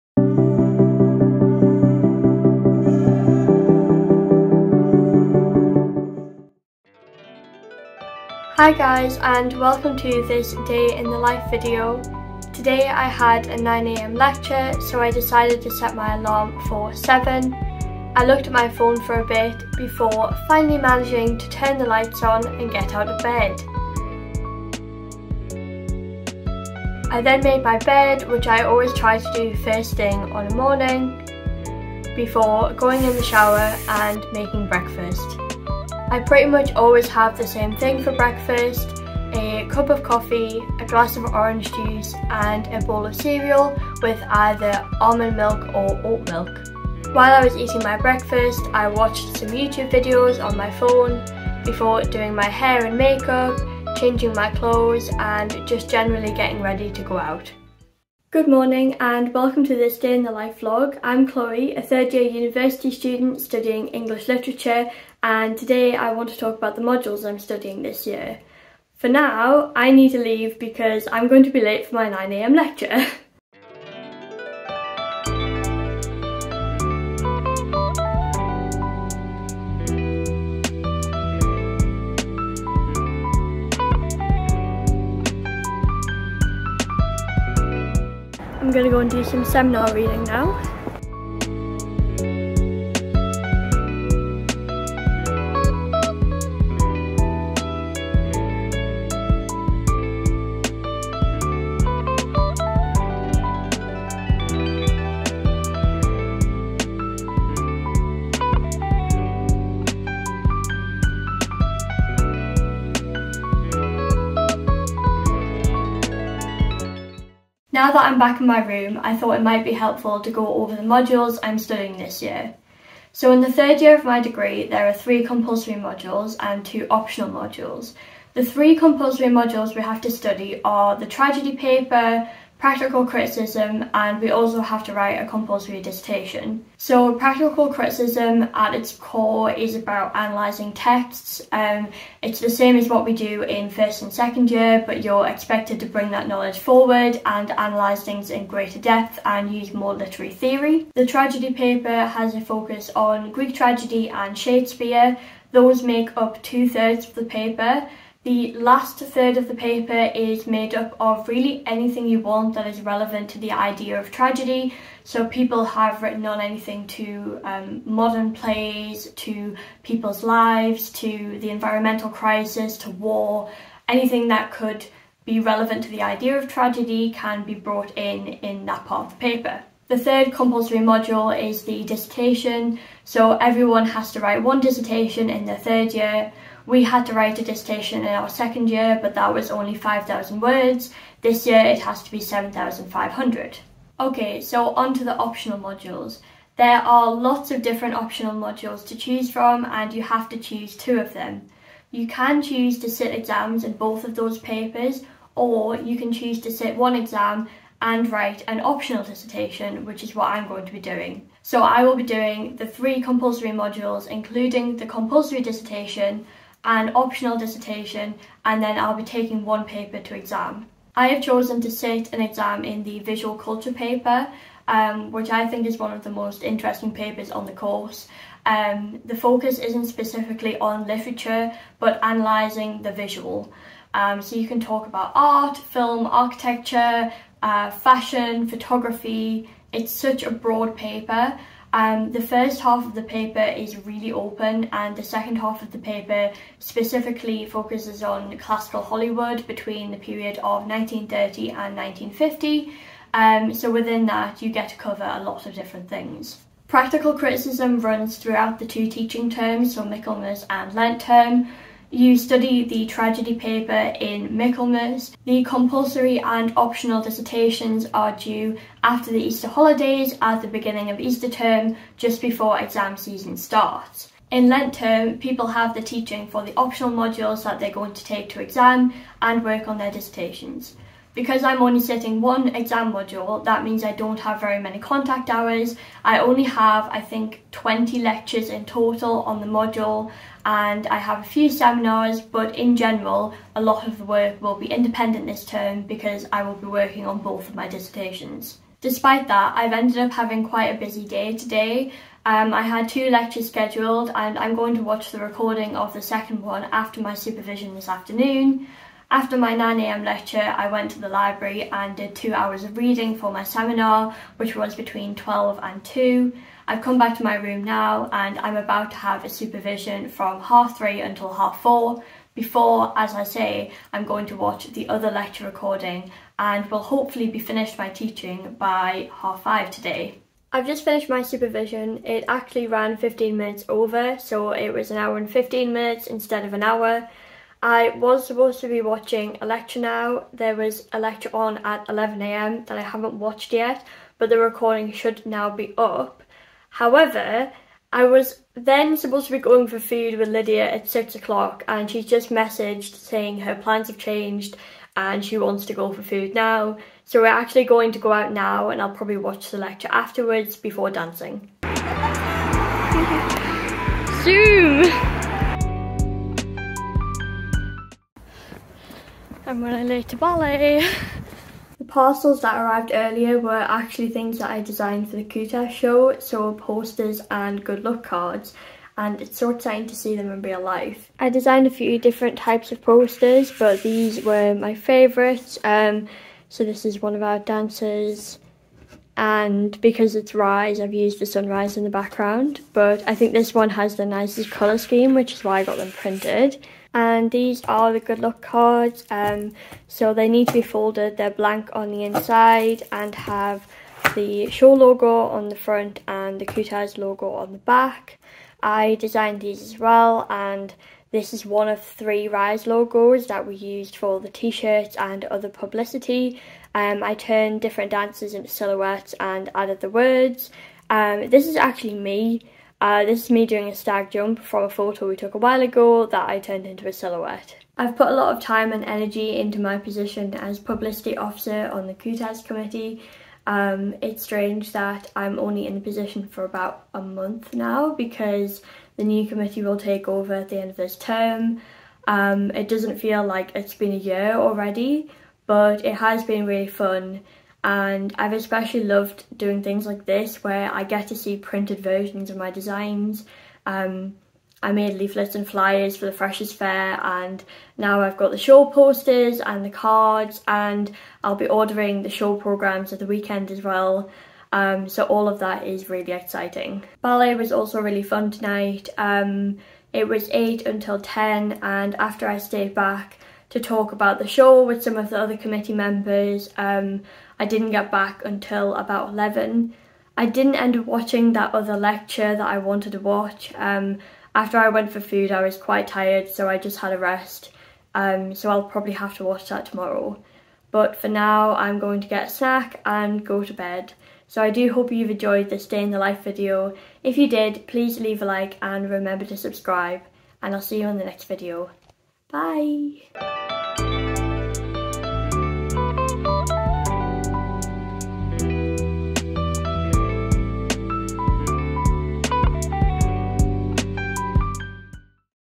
A Day in the Life of a Cambridge Student 📚 What does a 3rd Year English Literature Student Study?
Hi guys, welcome to this day in the life of a Cambridge University English Literature Student. Join me for a day of lectures, supervisions and studying, while I explain what an English Literature student in 3rd year studies. I am currently working on my dissertations and an exam paper called 'Visual Culture', and lent term is v busy and stressful!
Chloe x

I have lots of exciting vlogs to edit over the next few weeks but I'm v busy editing my dissertations currently - subscribe and turn on notifications so you don't miss my next video! :))

Filmed: Lent Term Week 1